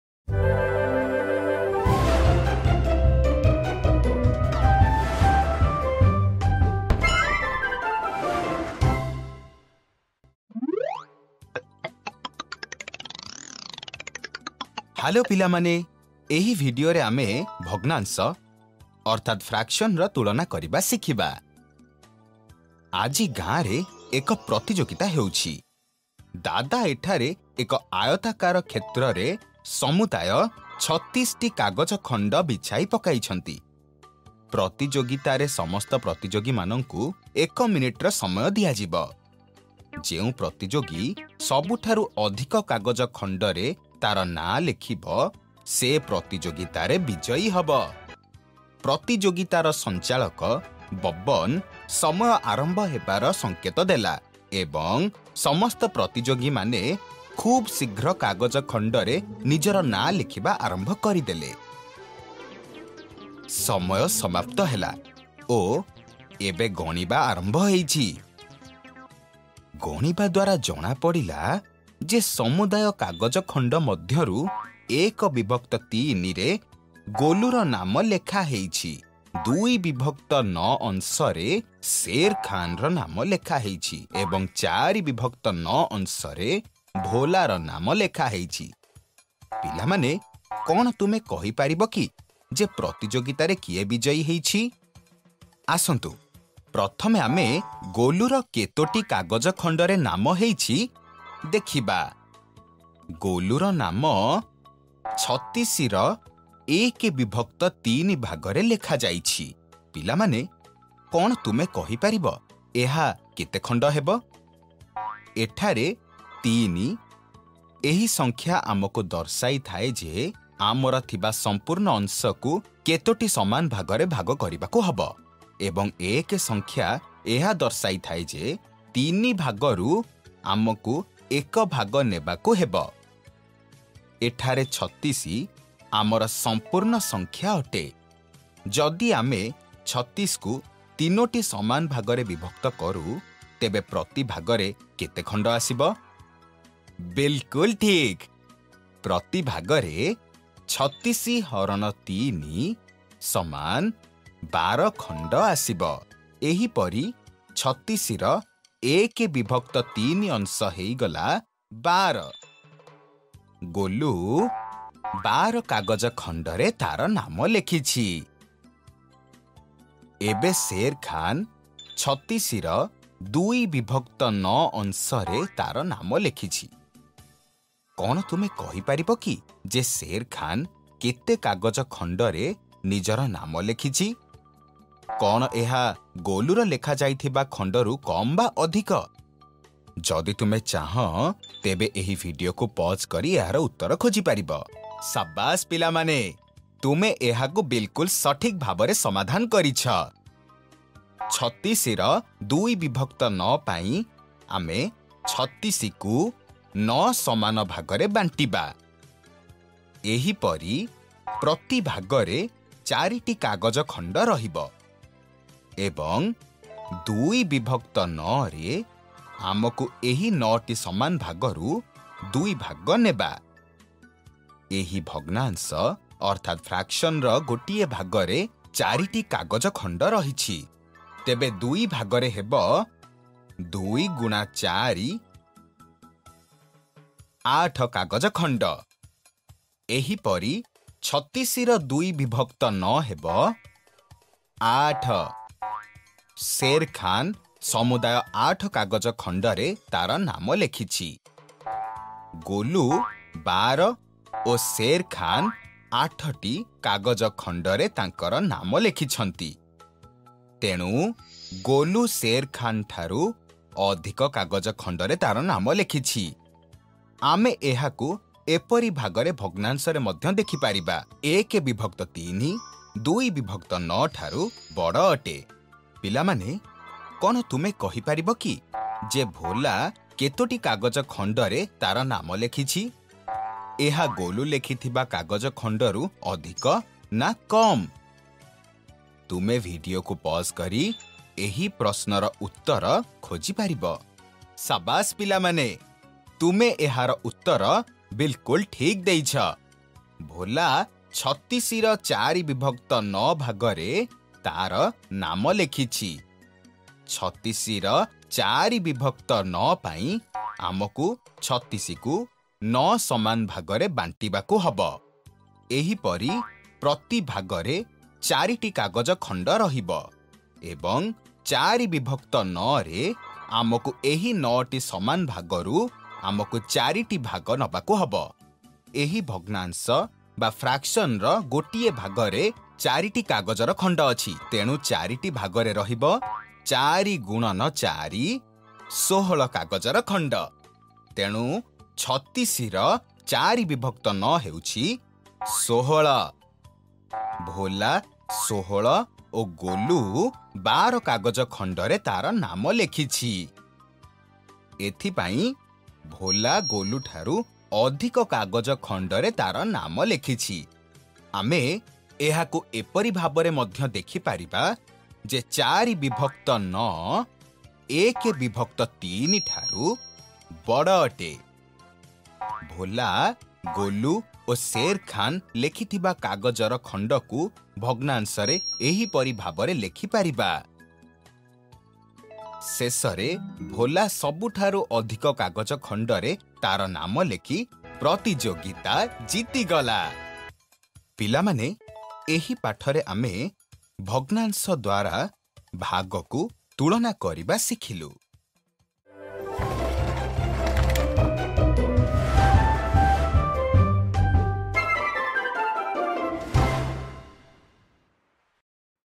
ଭଗ୍ନାଂଶଗୁଡ଼ିକର ତୁଳନା : ଭାଗ-1 | Part 1/3 | Comparison of fractions: Part 1 | Odia | Class 5
Class 5 | ଭଗ୍ନାଂଶଗୁଡ଼ିକର ତୁଳନା : ଭାଗ-1 | Part 1/3 | Comparison of fractions: Part 1 | Odia | Class 5

TicTacLearn Video
Interactive Videos